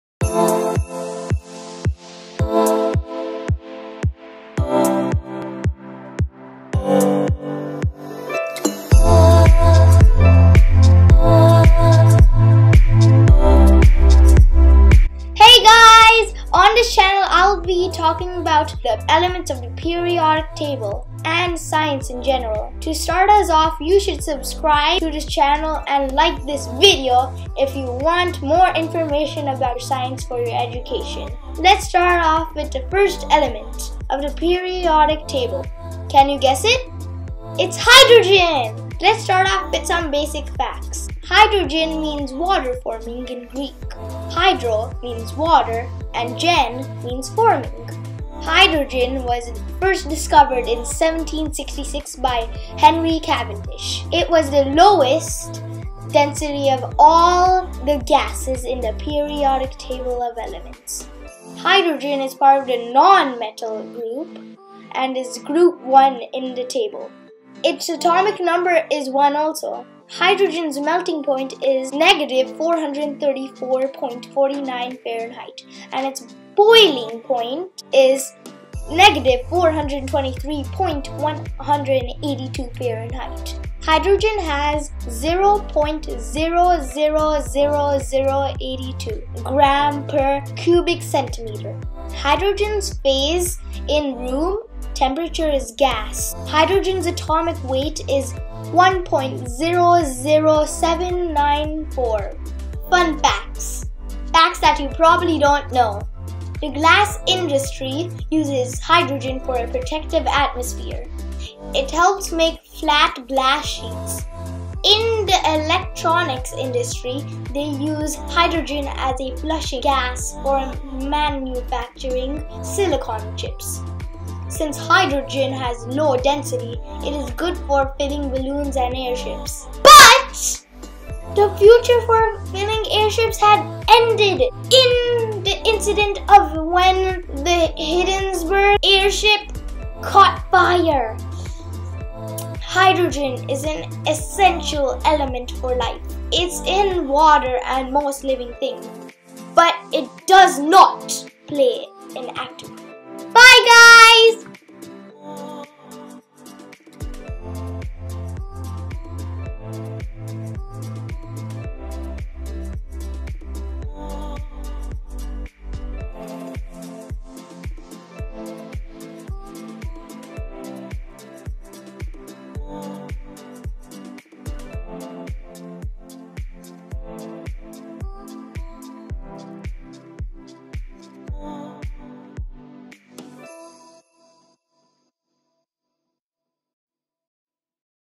What is Hydrogen? II The First Element Of The Periodic Table II Little Science Kid
The first video on this channel! On this channel I will be posting weekly videos on Fridays about the periodic table of elements and science in general! This video is the first element of the periodic table of elements which is. *drum roll please*. HYDROGEN!!! If you guys don't know what hydrogen is then I hope this video will help you! I'll see you guys in my next video, which will be next Friday! Bye for now! :D